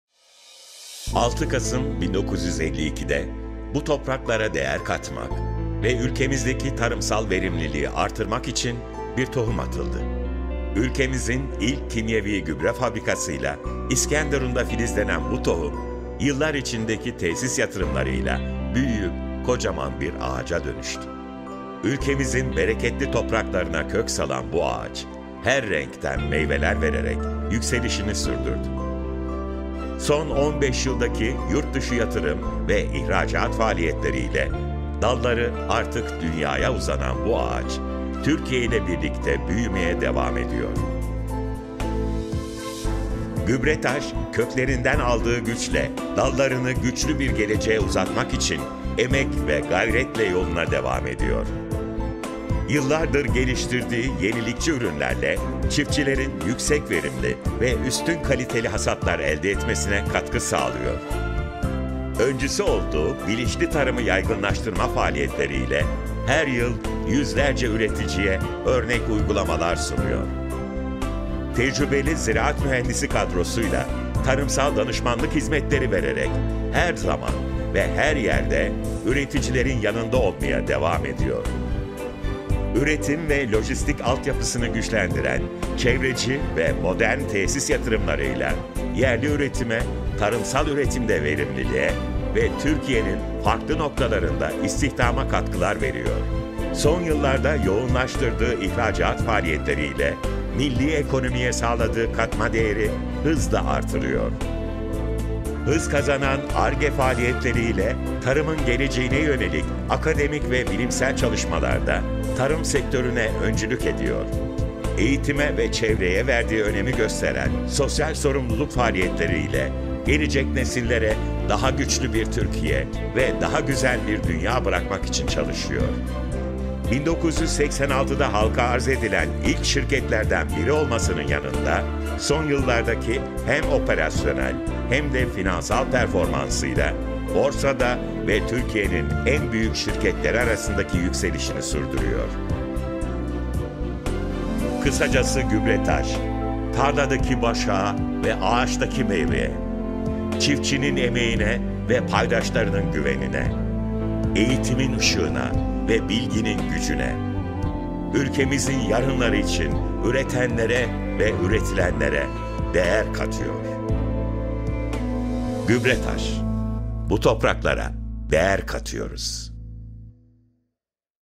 Bu Topraklara Değer Katıyoruz
6 Kasım 1952’de bu topraklara değer katmak ve ülkemizdeki tarımsal verimliliği arttırmak için bir tohum atıldı.
Ülkemizin ilk kimyevi gübre fabrikasıyla İskenderun’da filizlenen bu tohum, yıllar içindeki tesis yatırımlarıyla büyüyüp kocaman bir ağaca dönüştü.
Ülkemizin bereketli topraklarına kök salan bu ağaç, her renkten meyveler vererek yükselişini sürdürdü.
Son 15 yıldaki yurtdışı yatırım ve ihracat faaliyetleriyle dalları artık dünyaya uzanan bu ağaç, Türkiye ile birlikte büyümeye devam ediyor.

GÜBRETAŞ, köklerinden aldığı güçle dallarını daha güçlü bir geleceğe uzatmak için emek ve gayretle yoluna devam ediyor...

Yıllardır geliştirdiği yenilikçi ürünlerle çiftçilerin yüksek verimli ve üstün kaliteli hasatlar elde etmesine katkı sağlıyor.

Öncüsü olduğu bilinçli tarımı yaygınlaştırma faaliyetleriyle her yıl yüzlerce üreticiye örnek uygulamalar sunuyor.

Tecrübeli ziraat mühendisi kadrosuyla tarımsal danışmanlık hizmetleri vererek her zaman ve her yerde üreticilerin yanında olmaya devam ediyor.

Üretim ve lojistik altyapısını güçlendiren çevreci ve modern tesis yatırımlarıyla yerli üretime, tarımsal üretimde verimliliğe ve Türkiye’nin farklı noktalarında istihdama katkılar veriyor.

Son yıllarda yoğunlaştırdığı ihracat faaliyetleriyle milli ekonomiye sağladığı katma değeri hızla artırıyor.

Hız kazanan Ar-Ge faaliyetleriyle tarımın geleceğine yönelik akademik ve bilimsel çalışmalarda tarım sektörüne öncülük ediyor.

Eğitime ve çevreye verdiği önemi gösteren sosyal sorumluluk faaliyetleriyle gelecek nesillere daha güçlü bir Türkiye ve daha güzel bir dünya bırakmak için çalışıyor.

1986’da halka arz edilen ilk şirketlerden biri olmasının yanında son yıllardaki hem operasyonel hem de finansal performansıyla Borsa’da ve Türkiye’nin en büyük şirketleri arasındaki yükselişini sürdürüyor.

Kısacası GÜBRETAŞ;
tarladaki başağa ve ağaçtaki meyveye,
çiftçinin emeğine ve paydaşlarının güvenine,
eğitimin ışığına ve bilginin gücüne,
ülkemizin yarınları için üretenlere ve üretilenlere
DEĞER KATIYOR!